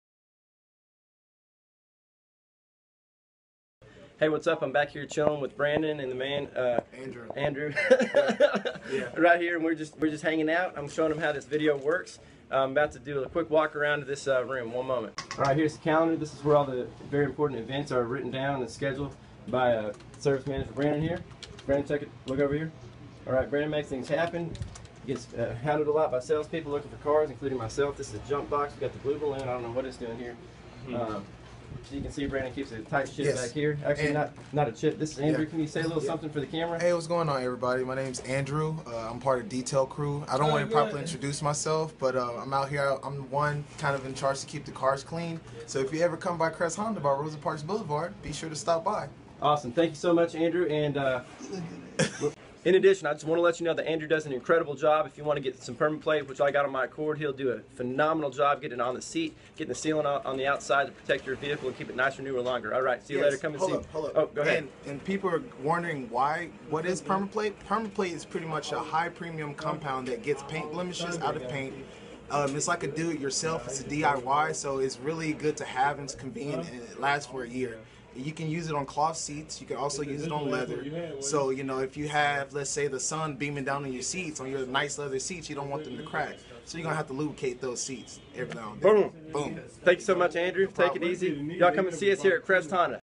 Behind the scenes at Crest Honda!
Feat. Andrew Lee Detail Specialist and Service Manager Brandon Moore!